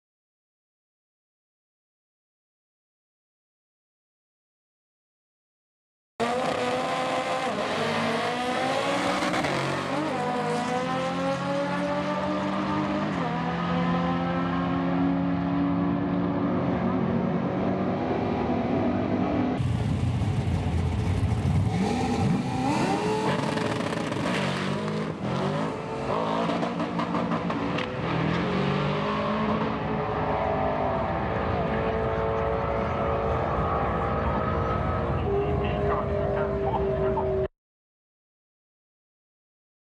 Night Of Thrills At The Track
Having a little wheels up from at the race track.